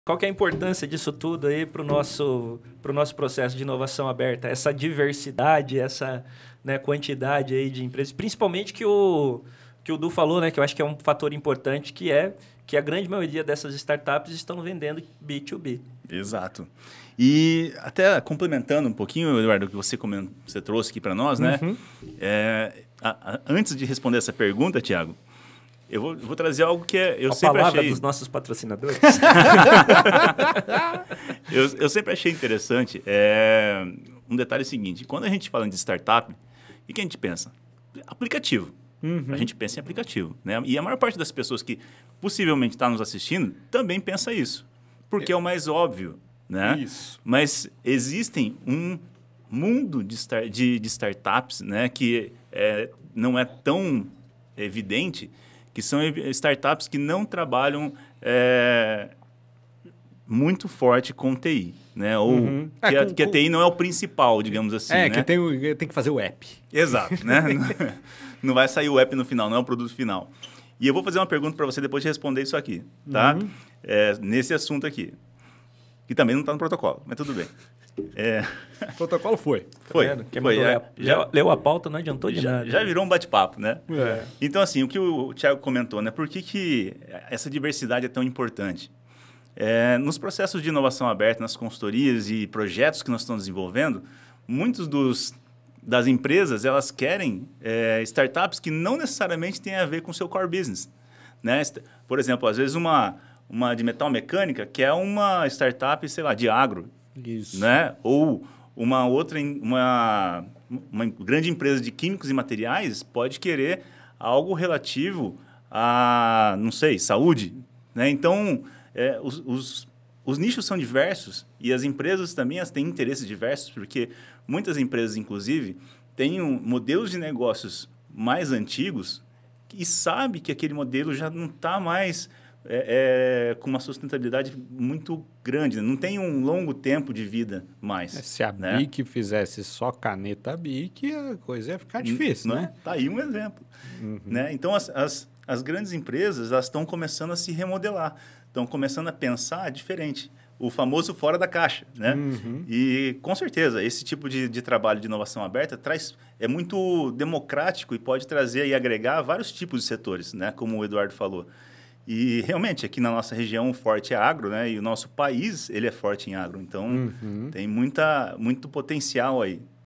QUAL A IMPORTÂNCIA DA DIVERSIDADE PARA O PROCESSO DE INOVAÇÃO ABERTA
Seu novo Canal de Conteúdo Sobre Inovação Aberta !!
Conheça também a Comunidade Sebrae sua plataforma de Conhecimento em Gestão e Negócios acesse